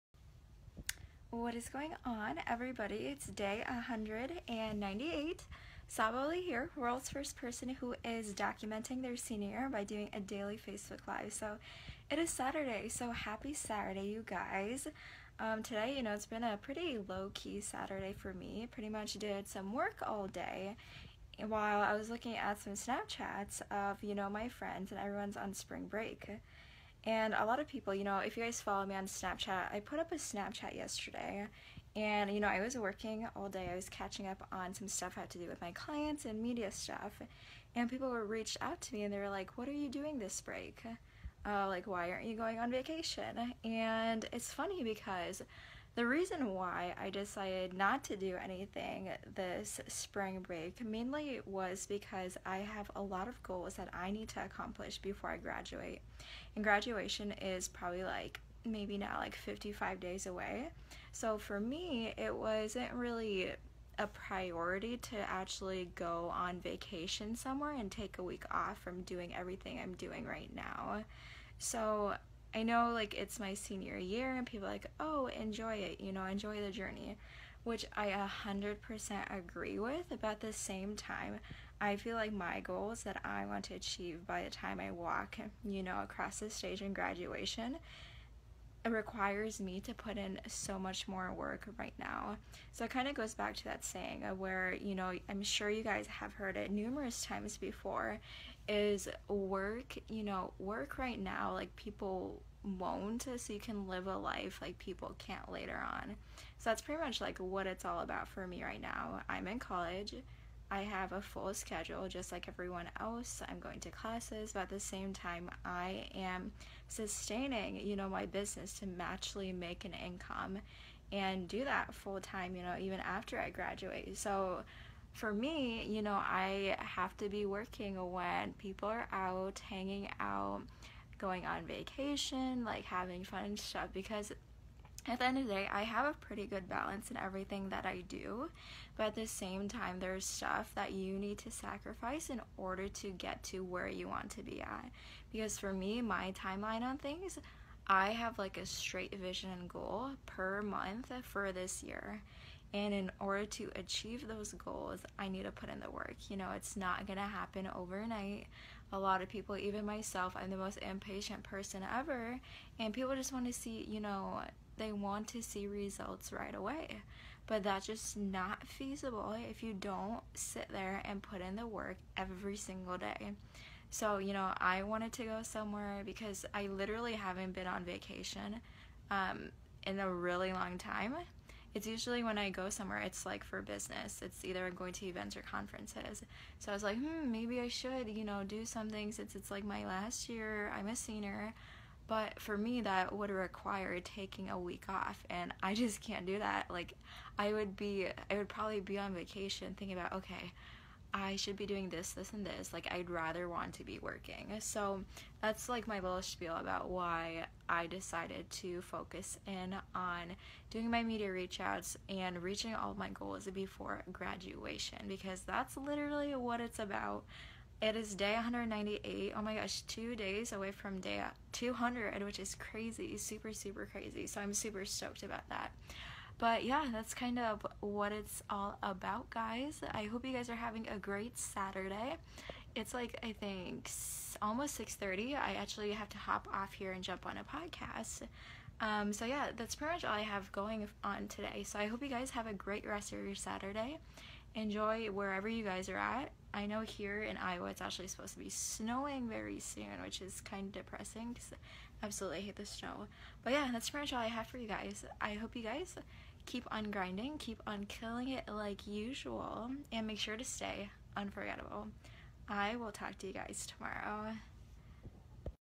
DAY 198: Everyone is on vacation for spring break!
Why am I not? Good question... tune in to listen to why I decided not to take a week off!